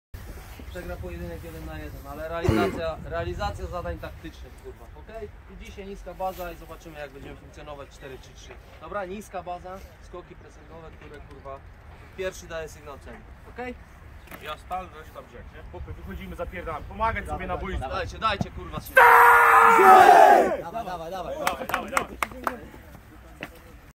Sparing Stal Brzeg - WKS Śląsk II Wrocław 12-02-2022
Drużyna Stali Brzeg przed rozpoczęciem meczu kontrolnego z rezerwami Śląska Wrocław. Mecz odbył się we Wrocławiu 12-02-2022